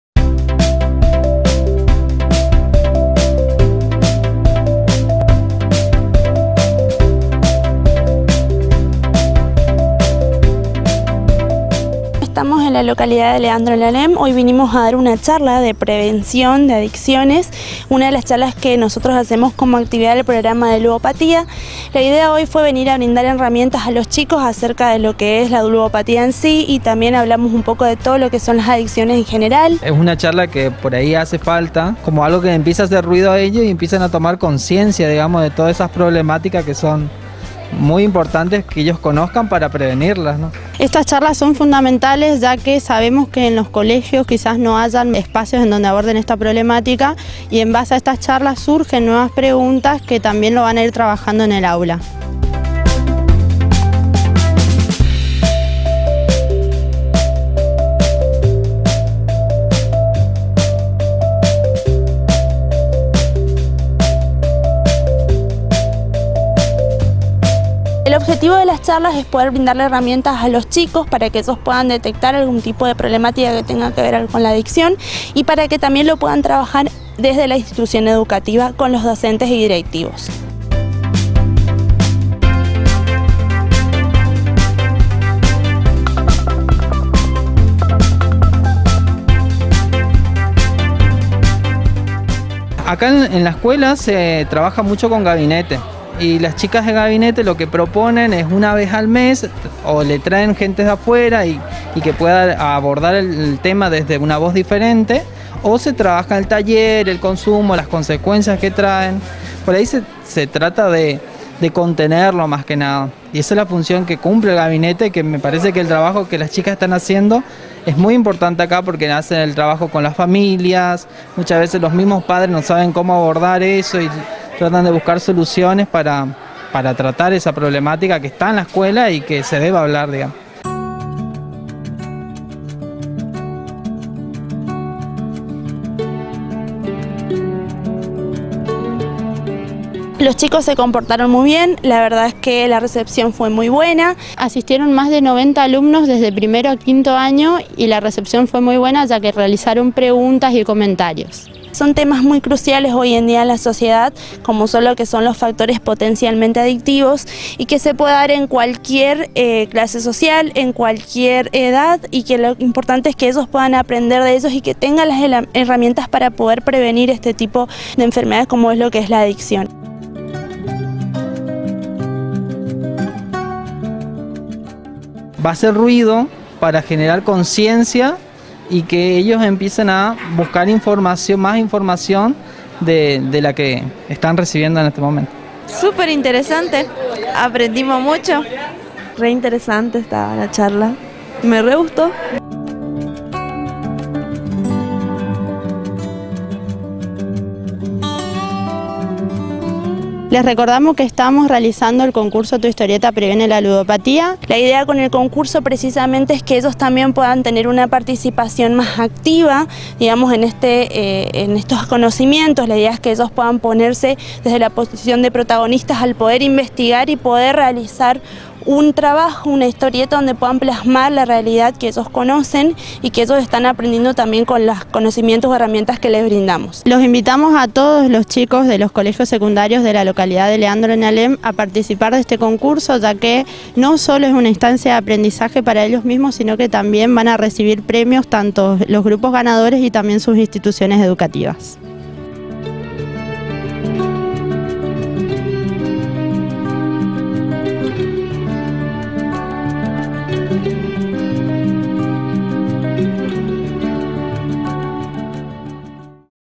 Área de Ludopatía - Charla en Alem
Charla de prevención de adicciones junto al área de Ludopatía en el Bachillerato Pedagogico Nº3 de Alem, en el marco del concurso tu historieta previene la ludopatia y como parte de su cronograma. Asistieron alrededor de 100 alumnos de nivel secundario.